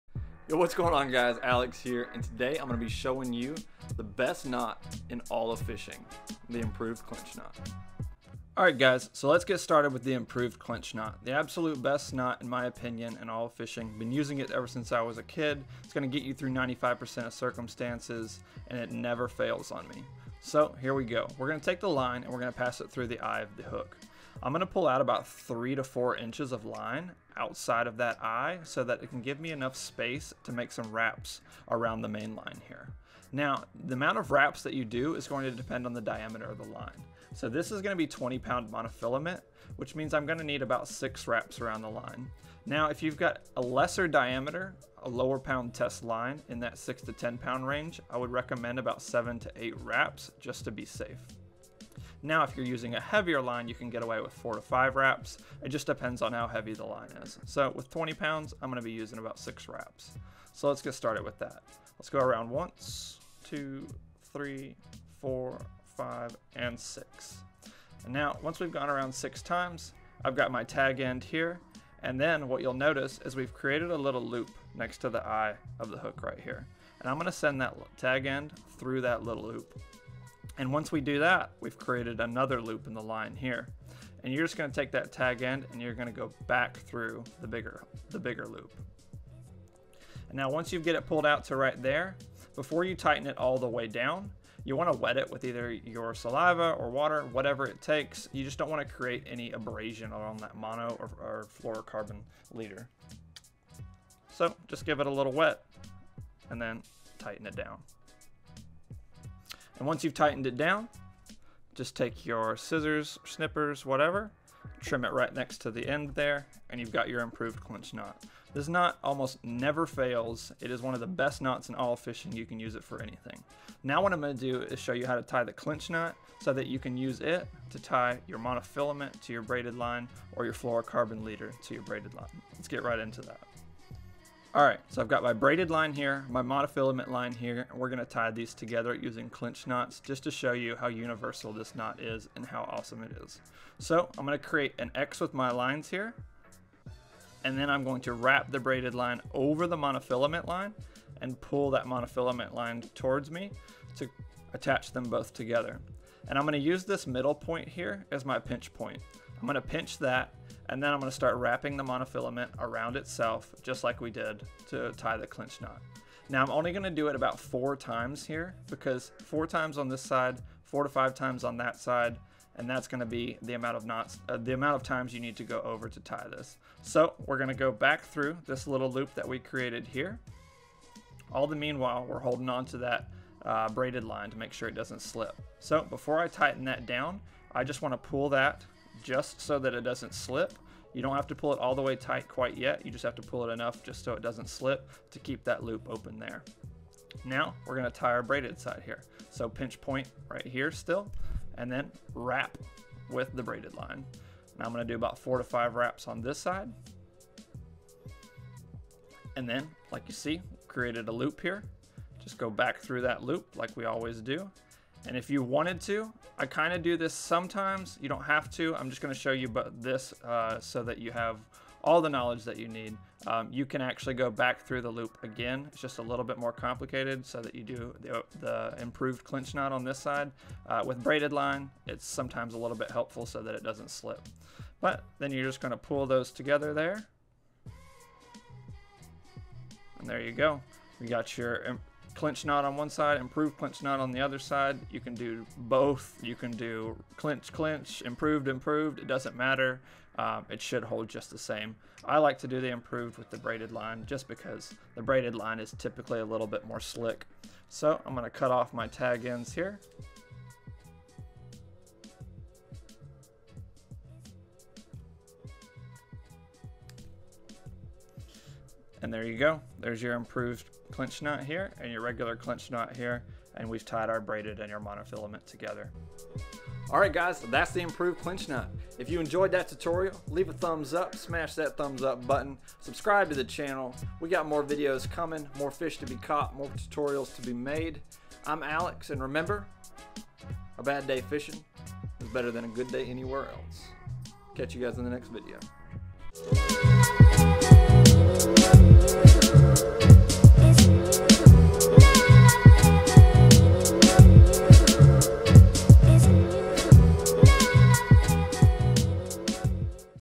The Improved Clinch Knot [BEST KNOT!]
The clinch and improved clinch knot are two of the most universal and best fishing knots out there. If I had to pick one knot to use for everything - it would be this one.

Comments are open for any questions you might have!

"A bad day fishing beats a good day anywhere else."

►►►FISHING GEAR USED IN IN VIDEO◄◄◄

►Power Pro Braided Line
► Big Game 20 lb. Mono

►►►RECORDING EQUIPMENT USED IN VIDEO◄◄◄

► Canon G7X Mark II
► Joby GorillaPod